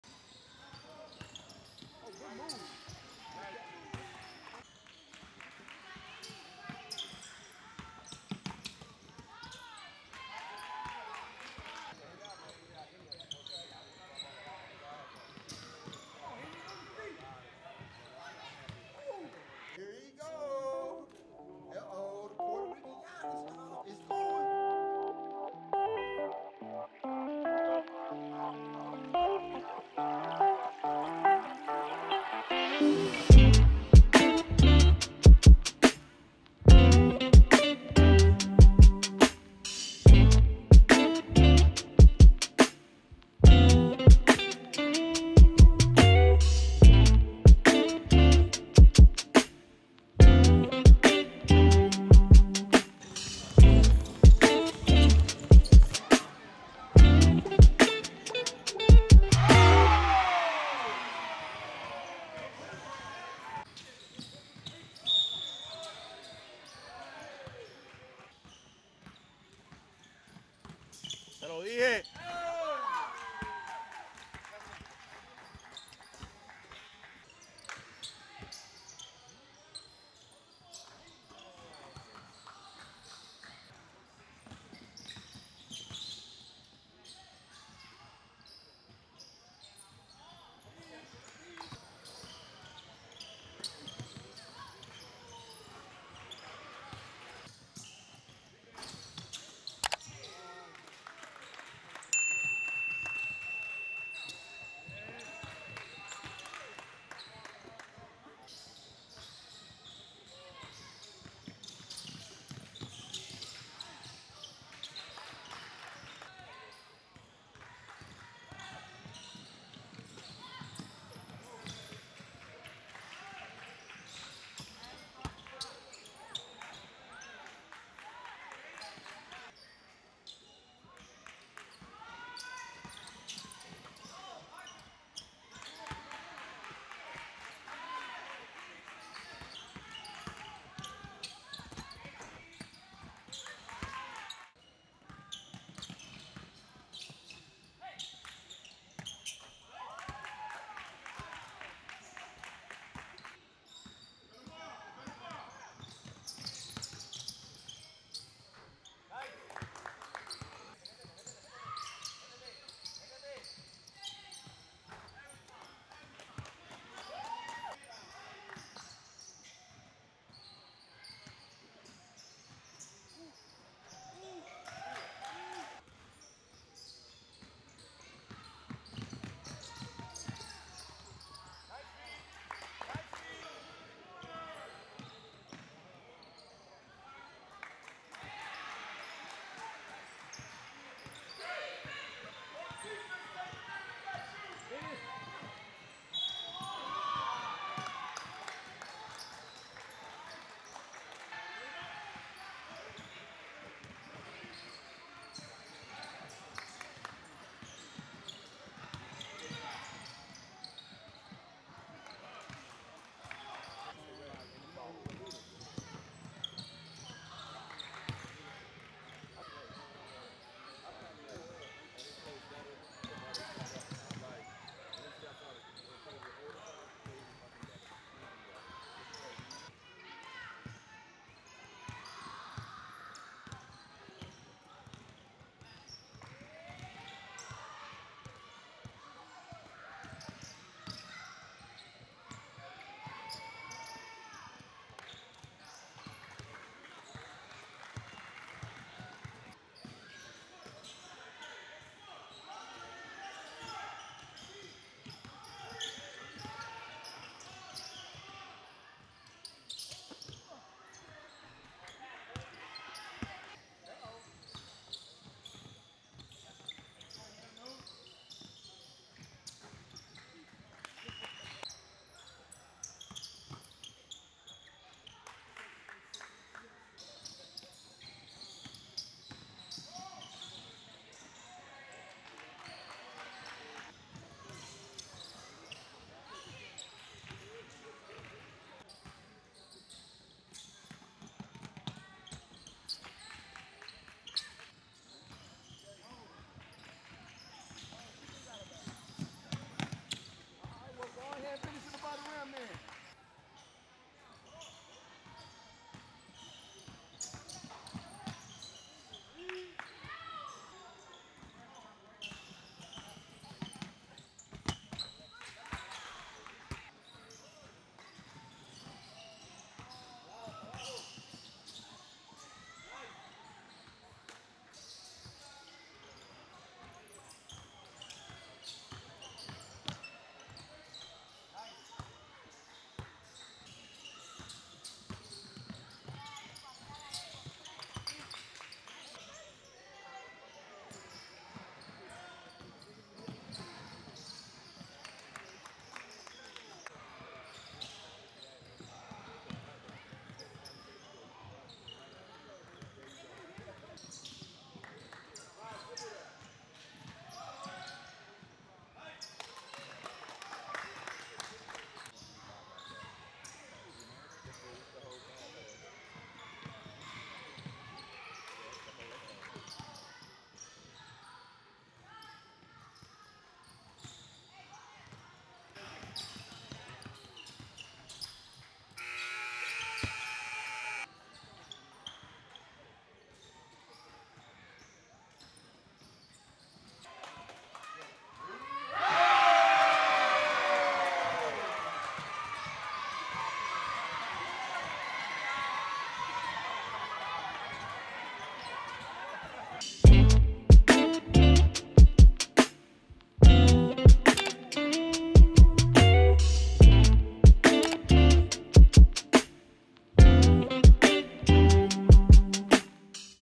7th Grader FELIPE QUINONES DUNKING at the TALENT OVER HYPE Mc DAVID ELITE CAMP 2021
First time playing at the DME Sports Academy in Daytona Beach, FL. Great weekend in town at the Talent Over Hype Camp powered by Elite Camps USA & Mc David USA.